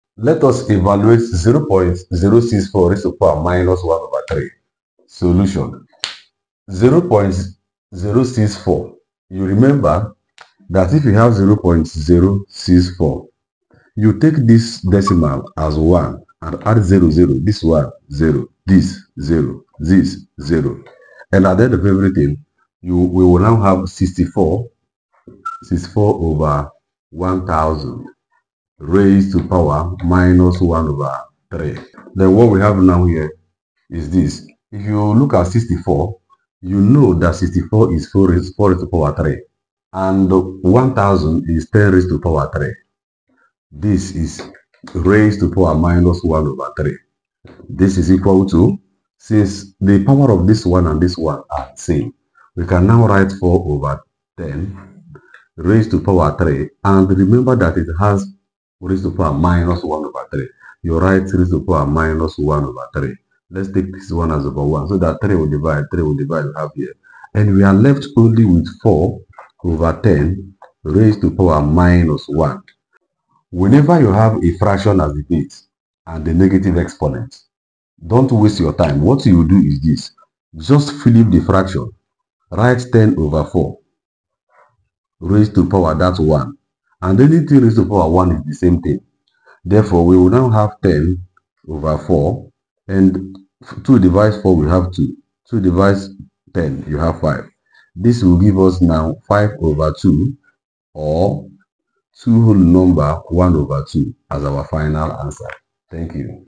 How to solveExponential Equations with a decimal base
Learn how to solve exponential equations easily with clear, step-by-step methods! 💡
Understand the concept, shortcuts, and tricks to tackle exponential problems fast.
Perfect for students, teachers, and anyone preparing for math exams.